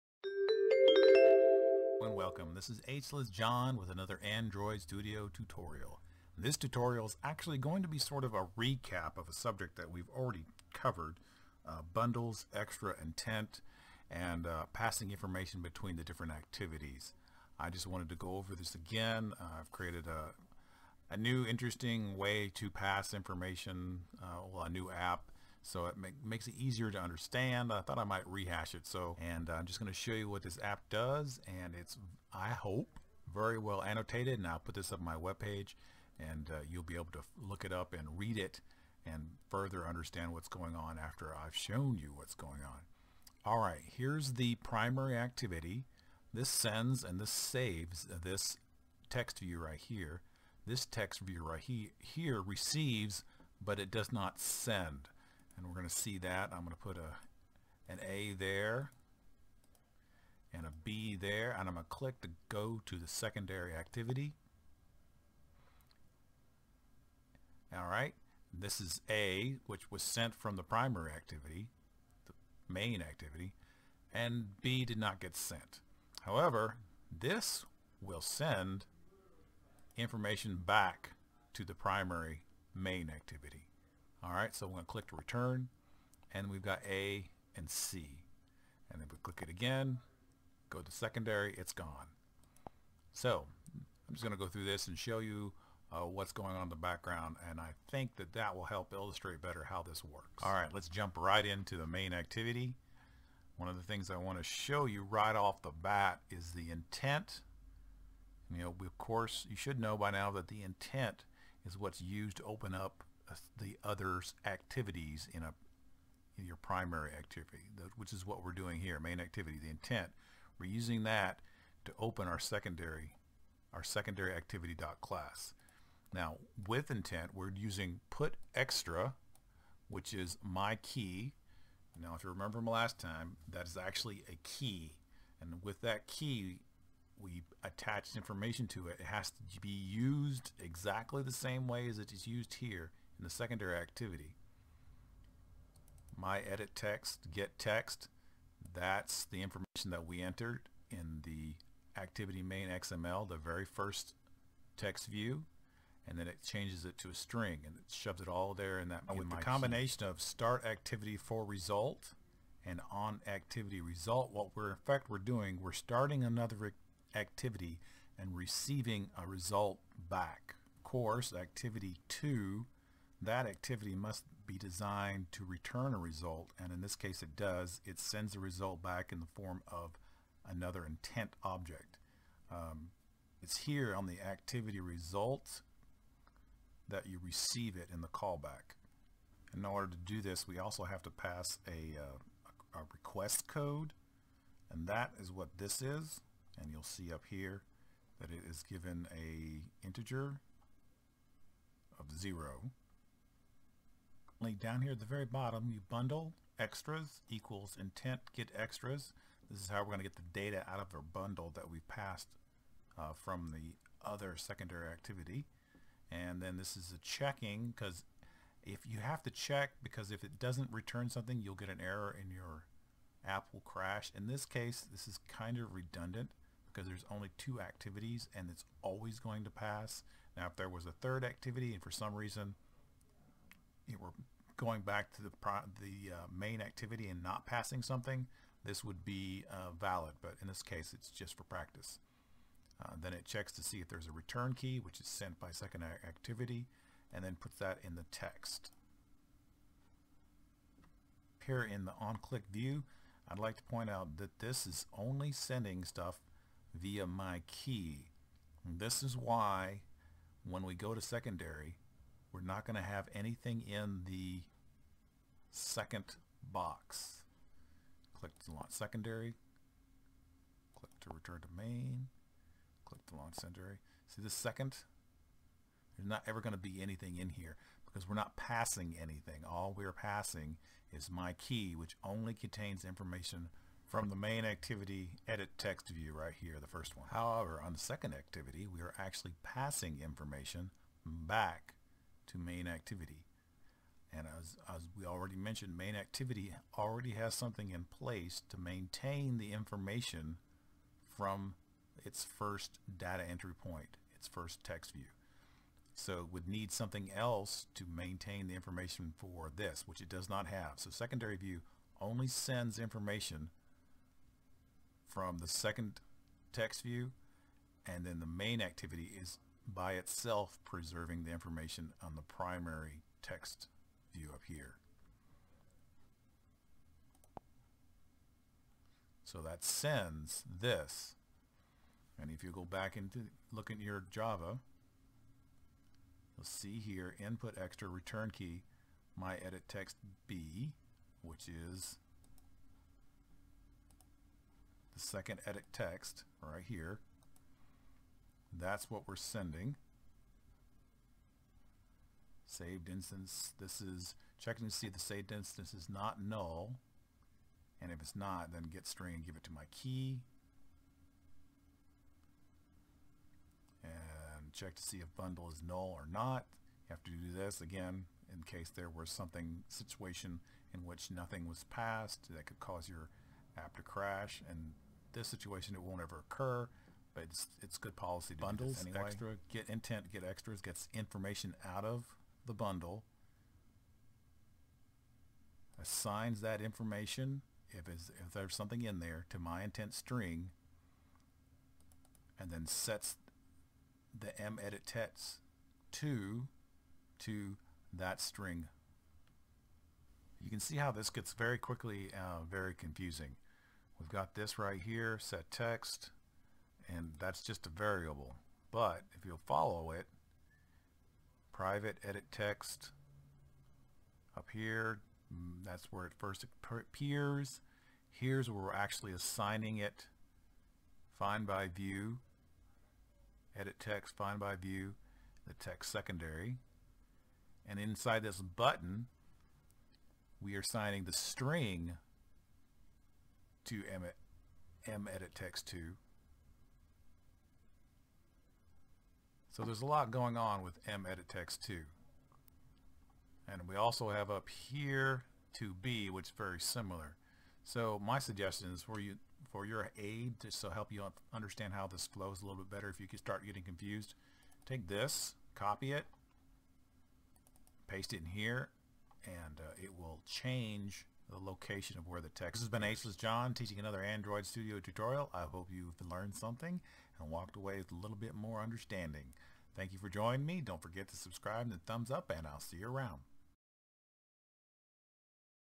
Beginners Android Studio Tutorial – 47 Bundle, Extra, and Intent: Data Transfer
Beginners Android Studio Tutorial – 47 Bundle, Extra, and Intent: Data Transfer.  Opening up a topic already covered and discussing it again.  Data transfer seems to be an interest area that many people have problems with, so there will be a few videos here working with different aspects of data transfer.

Other Resources:
Getting a Result from an Activity